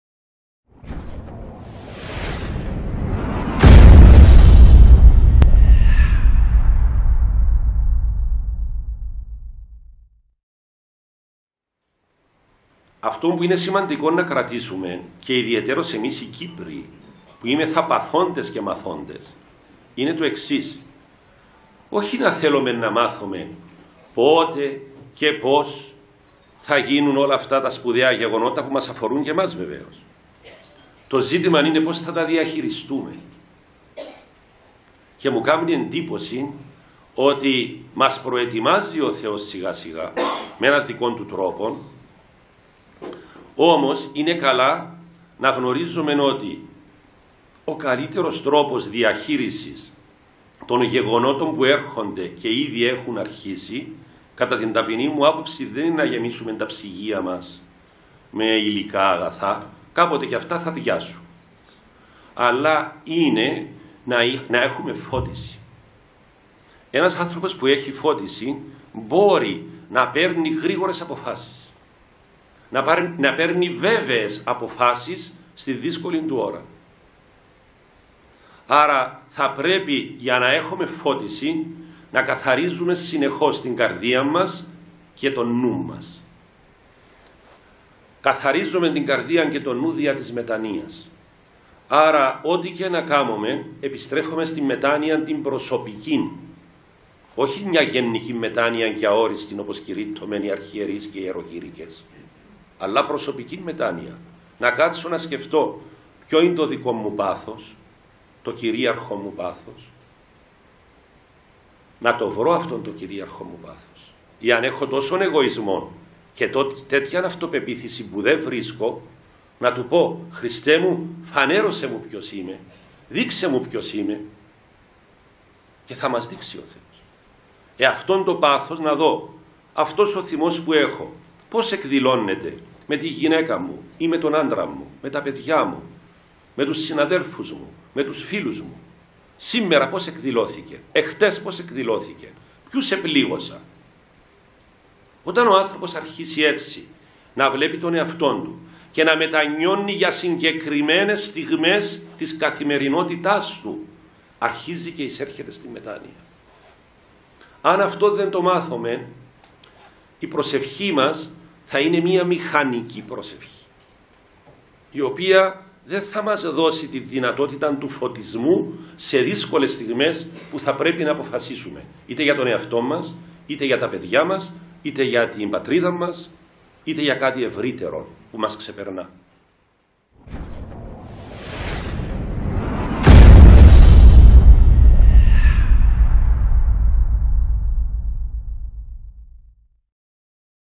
''Μετάνοια'' Ο καλύτερος τρόπος διαχείρισης των γεγονότων που έρχονται (2015)
Απόσπασμα από την ομιλία που έδωσε ο Μητροπολίτης Μόρφου Νεόφυτος, μέσα από την οποία σκιαγράφησε την προσωπικότητα του Αγίου Παϊσίου του Αγιορείτου, έτσι όπως τον γνώρισε ο ίδιος. Η ομιλία πραγματοποιήθηκε στην αίθουσα της Ενορίας Αποστόλου Βαρνάβα & Μακαρίου Δασούπολης σε εκδήλωση που διοργανώθηκε από την Ενορία Αποστόλου Ανδρέα Συνοικισμού ΣΤΡΟΒΟΛΟΣ 3 Λευκωσίας Κύπρου [2015].